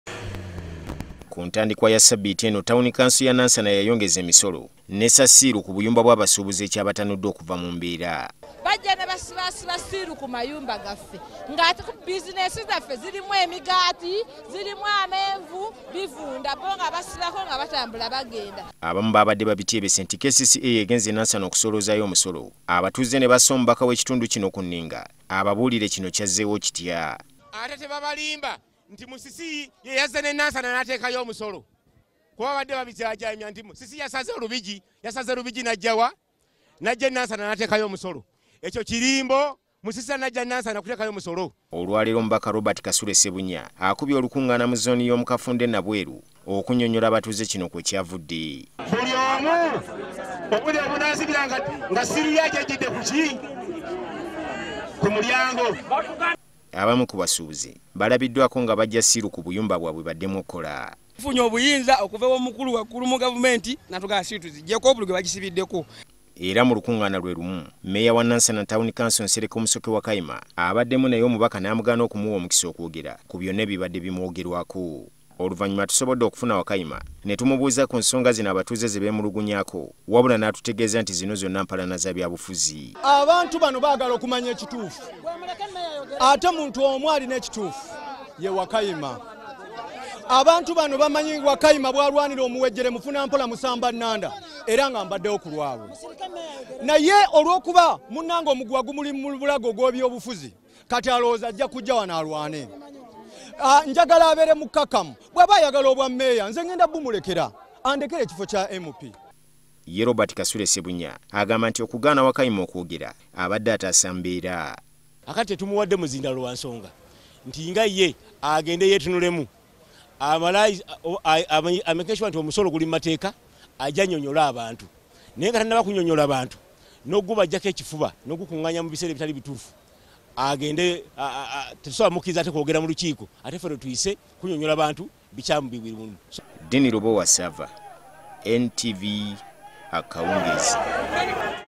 Emisolo e Nansana: Meeya alemeseddwa okwewozaako
Omubaka wa Kyadondo North, Robert Kasule Ssebunya, atabukidde Meeya wa Nansana Town Council n’amugaana okwogera mu lukungana lw’abadde akubye mubitundu bye Nabweru.